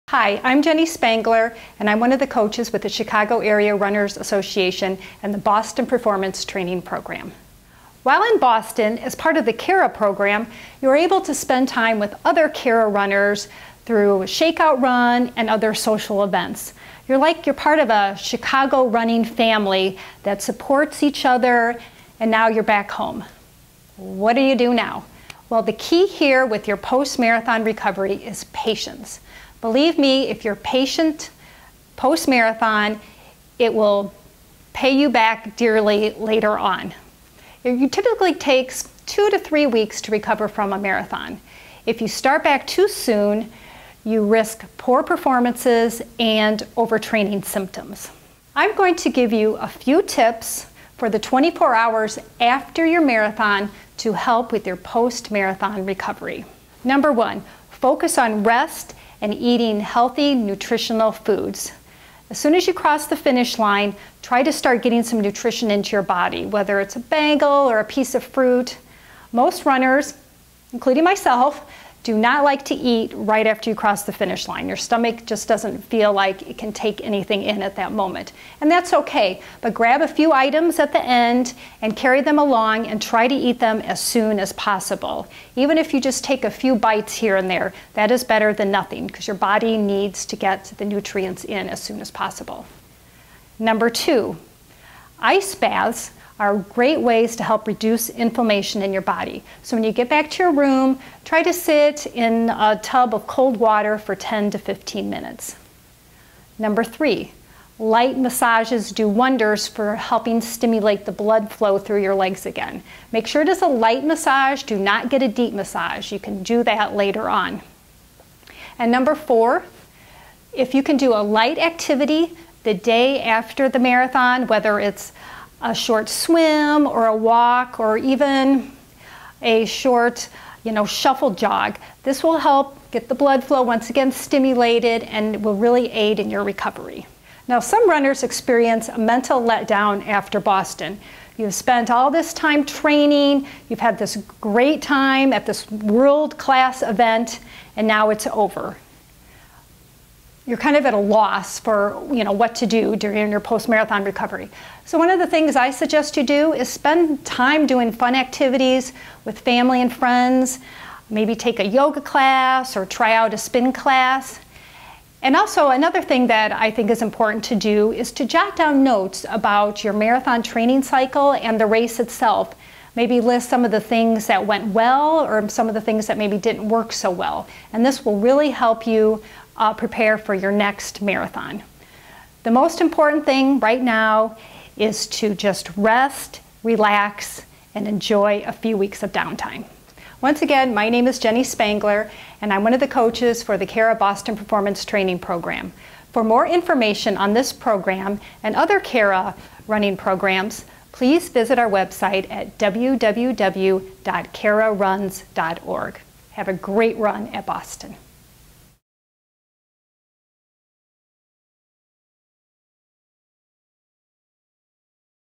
Coach Jenny Spangler Discusses Post Marathon Mental & Physical Recovery
Olympian Marathoner and Chicago Area Runners Association coach for the Boston Performance training group, Jenny Spangler, discusses post marathon do's and dont's. To register for training or for more information please